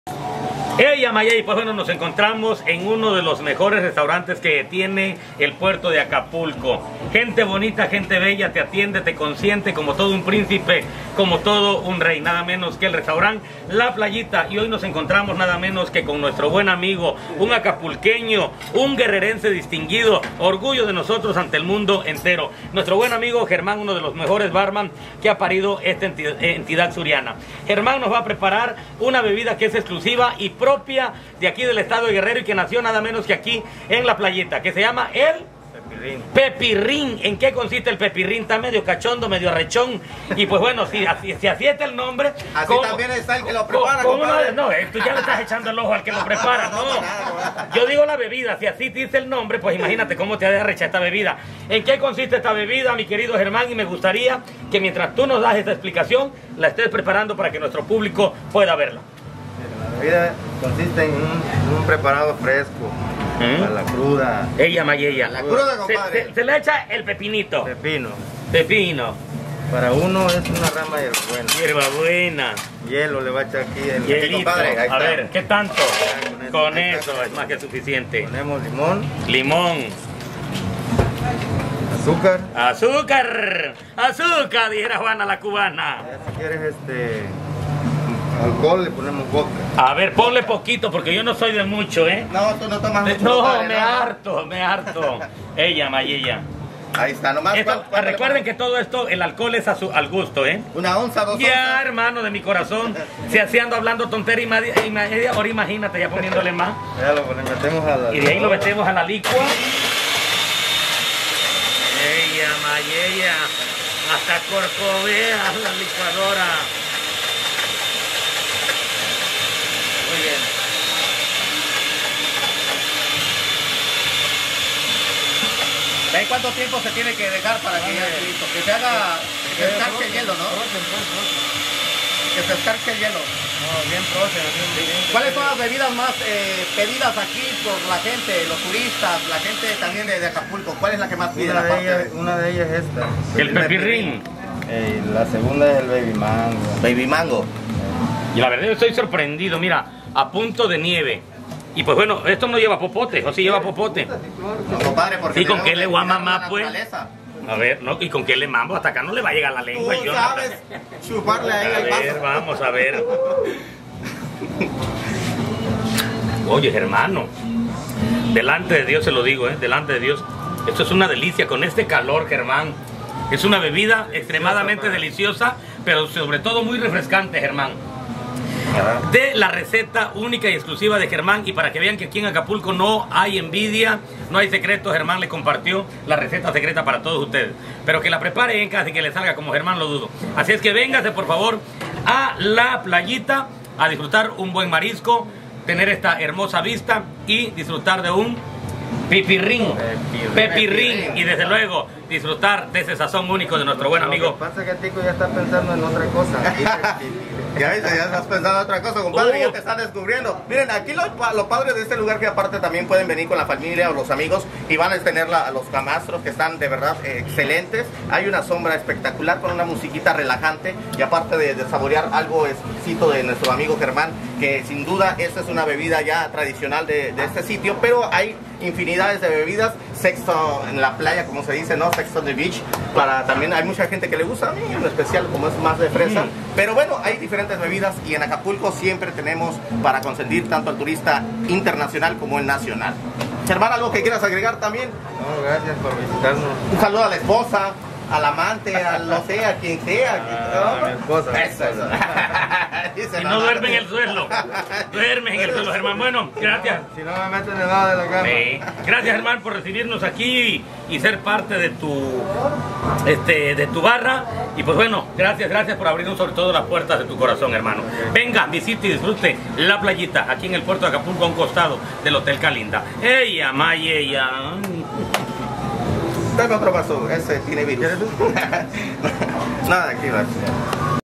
TICO MENDOZA PROBANDO EL PEPIRRÍN EN PLAYITA SANTA LUCÍA EN ACAPULCO
Tuve el honor de probar el Pepirrín en Playita Santa Lucía en Acapulco, soy su amigo Tico Mendoza El Arrecho de la Costa.